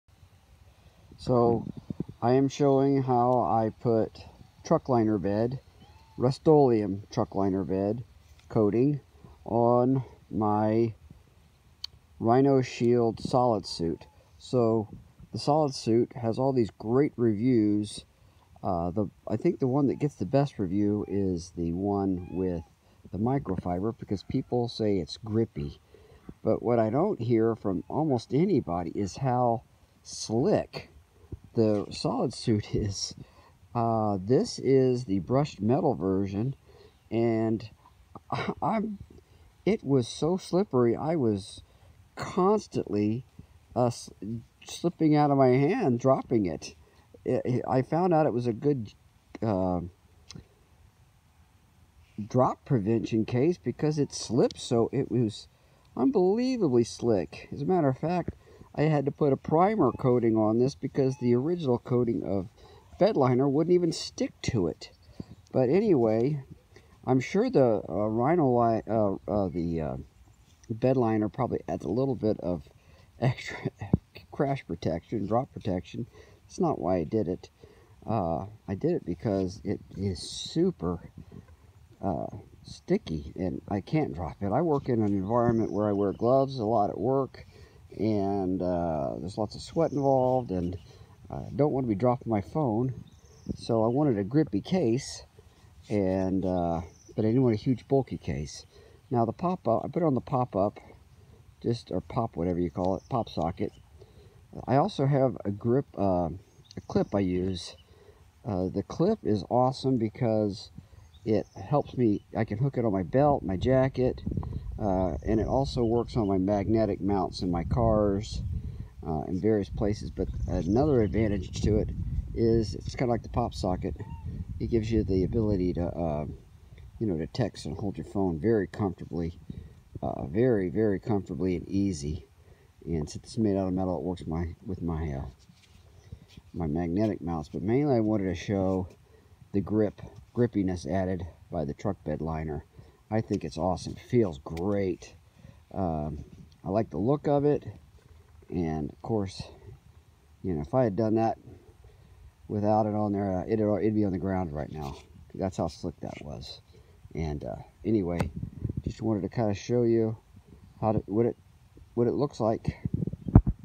Truck bed liner hack cell phone case rhinoshield solidsuit
I think many cases lack good grip and the rhinoshield solidsuit is easily the slickest and least grippy cell phone case I've ever used (and that's a bunch!). Truck bed liner is a great, grippy coating to add extra grip to any cell phone case.